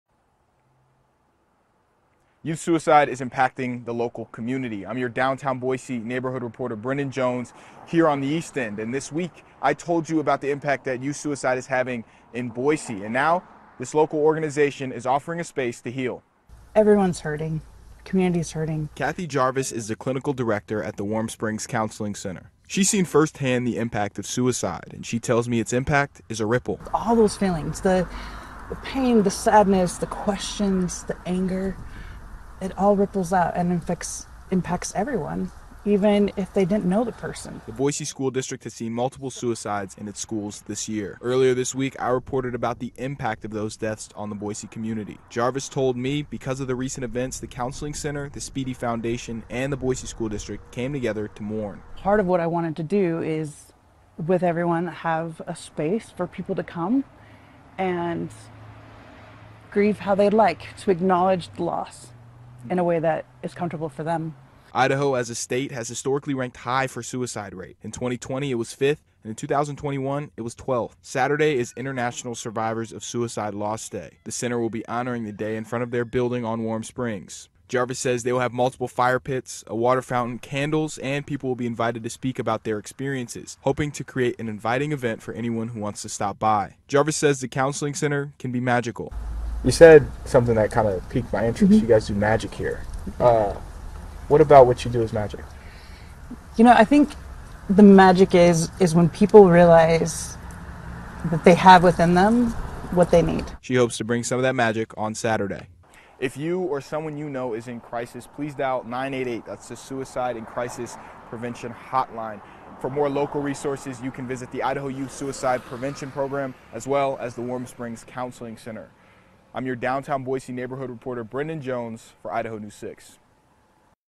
Raising suicide awareness. Warm Springs Counseling Center hosts mourning event
After multiple teen suicides struck the Boise community this year, the Warm Springs Counseling Center is hosting an event, giving people the space to mourn.